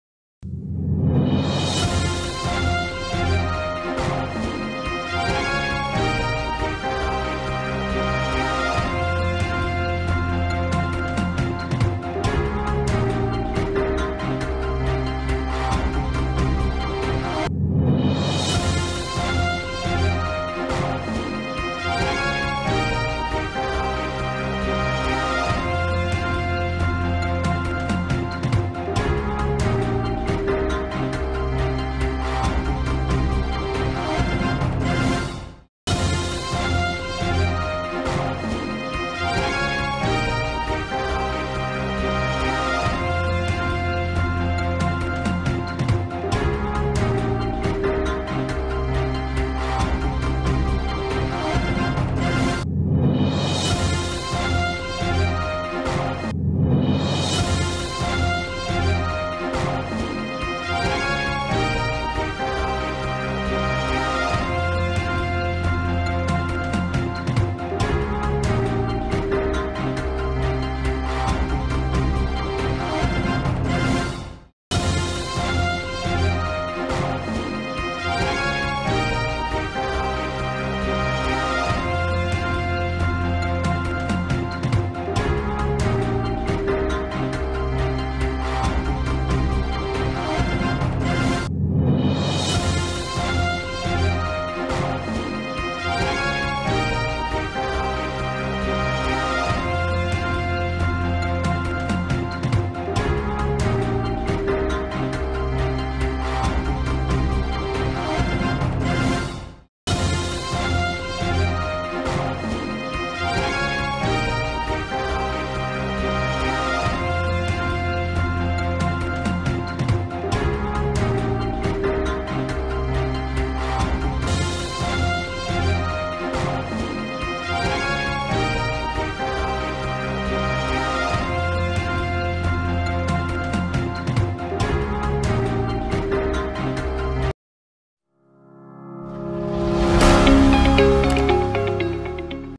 Delhi's Air Quality Dips To New Low Delhi Recorded More Than 700 AQI On 4th November....
With winter just around the corner, Delhi has once again been enveloped in a thick layer of smog as the air quality plummeted to 'severe' category. Here are some of the things that you need to know about the situation.